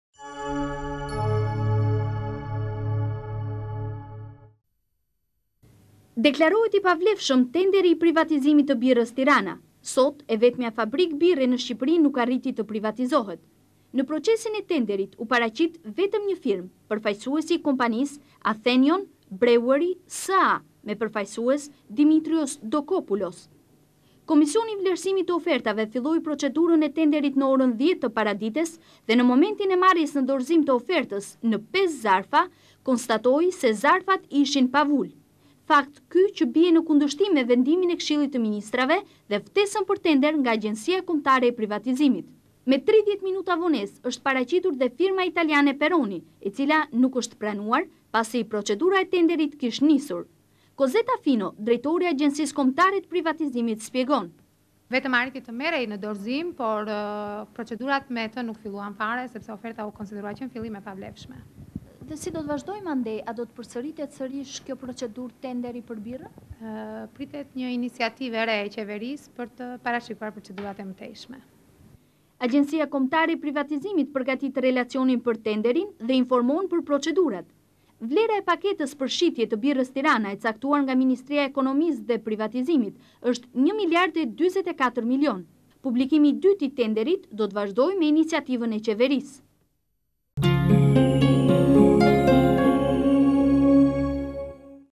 Birra Tirana nuk arrin te privatizohet (15 Gusht 2000)
Deklarohet i pavlefshëm tenderi i privatizimit të Birrës Tirana. Sot, e vetmja fabrikë birre në Shqipëri, nuk arriti të privatizohet. Në procesin e tenderit u paraqit vetëm një firmë. Përfaqësuesi i  kompanisë greke me përfaqësues Dimitrios Populos. Komisioni i Vlerësimit të Ofertave filloi procedurën e tenderit në orën 10:00 të paradites dhe në momentin e marrjes në dorëzim të ofertës në 5 zarfe, konstatoi se zarfet ishin pa vulë, fakt ky që bie në kundërshtim me vendimin e Këshillit të Ministrave dhe ftesën për tender nga Agjencia Kombëtare e Privatizimit.
Më 30 minuta vonesë është paraqitur dhe firma italiane Peroni e cila nuk është pranuar pasi procedura e tenderit kishte nisur. Kozeta Fino, drejtore e Agjensisë Kombëtare të Privatizimit, shpjegon:
Vetëm arriti të merrej në dorëzim, por procedurat nuk filluan fare pasi oferta u deklarua që  në fillim e pavlefshme. Pritet një iniciativë e re e qeverisë për të parashikuar procedurat e mëtejshme."
Agjencia Kombëtare e Privatizimit, përgatit relacionin për tenderin dhe informon për procedurat. Vlera e paketës për shitje të Birra Tirana e caktuar nga Ministria e Ekonomisë dhe Privatizimit është 1 miliard e 44 milion. Publikimi i dytë i tenderit do të vazhdojë me iniciativën e qeverisë./tvklan.al